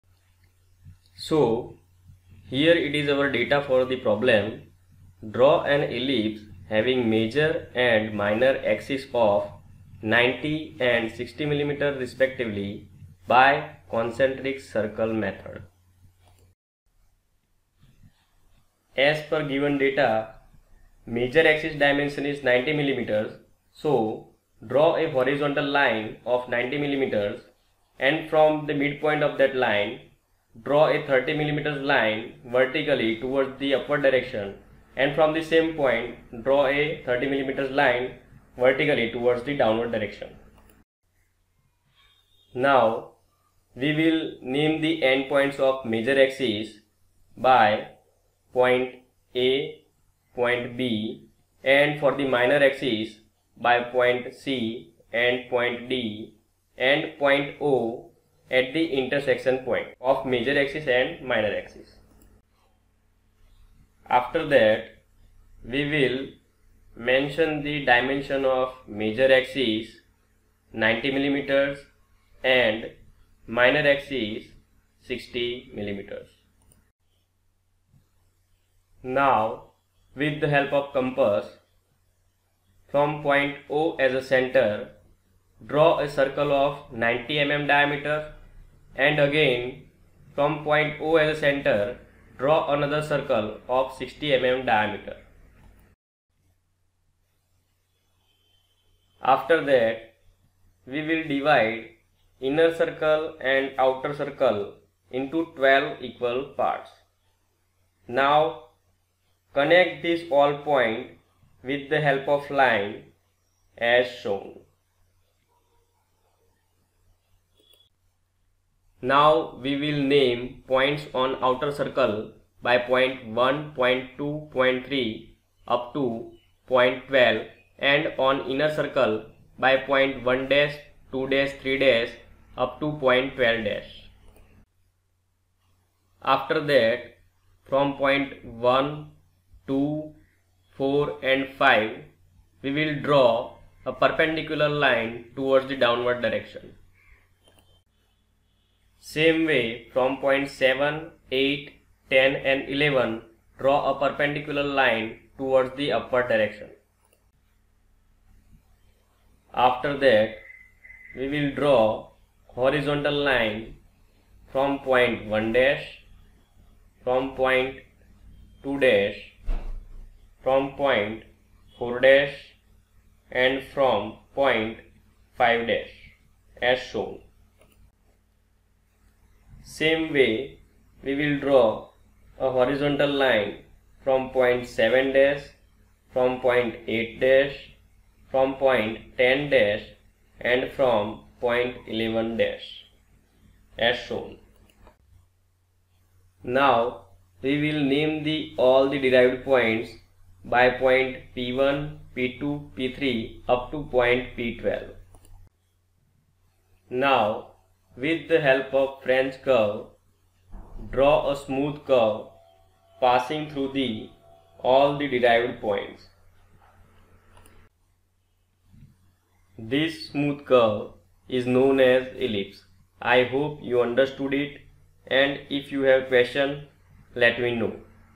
Ellipse by concentric circle method | Engineering Curves | Engineering Drawing
By watching this video you will be able to understand all the steps for drawing ellipse by the concentric circle method.

Instruments required to solve this problem are a roller scale, pencil, eraser, french curve, compass, and protractor.